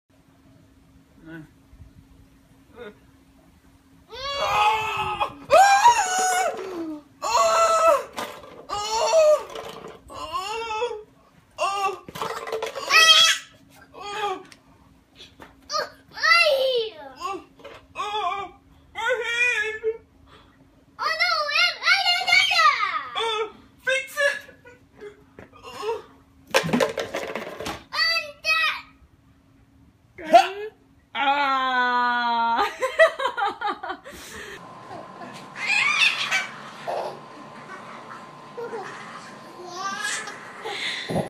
Dad Pranks Son With Fake Hand
See the fear in this kid's eyes when his dad tricks him into thinking he sawed off his own father's hand. Enjoy the years of therapy, fam!